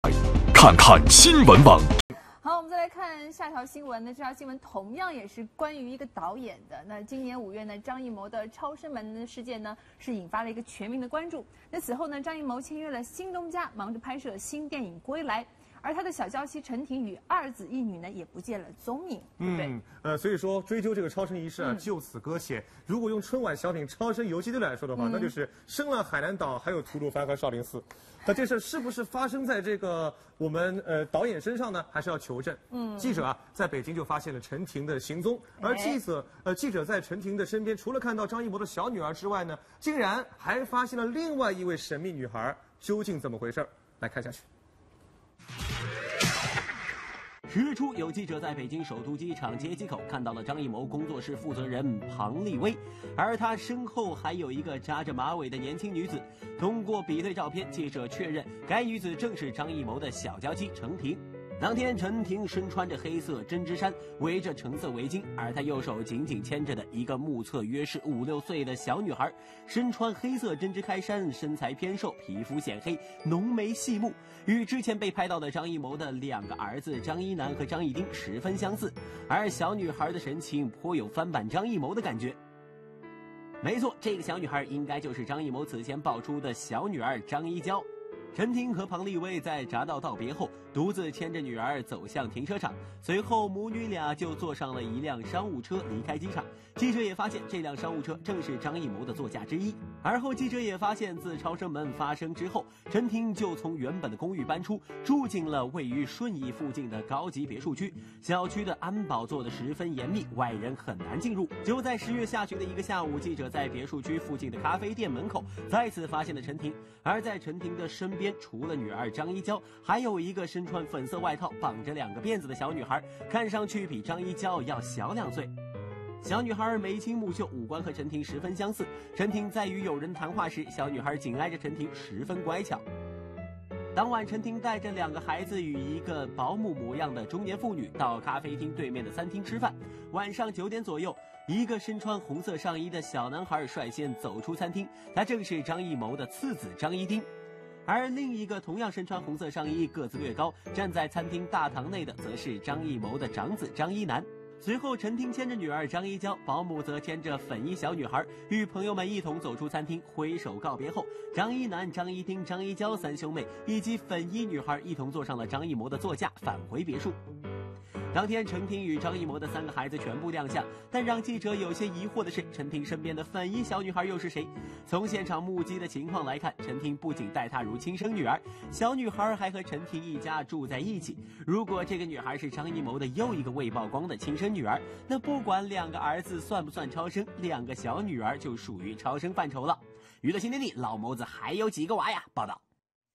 张艺谋小女儿首曝光 遭网友质问究竟有几个好儿女
今年5月初，张艺谋被曝"至少有7个孩子或面临1.6亿超生罚款"。江苏省无锡市计生委虽曾表态，称对张艺谋"超生"一事已开展调查，但现在时间过去了近半年时间却没有任何进展。昨日（11月3日），"南都娱乐周刊"官方微博首次曝光了张艺谋的小女儿，并称继张艺谋和陈婷有俩儿子外还有一个小女儿。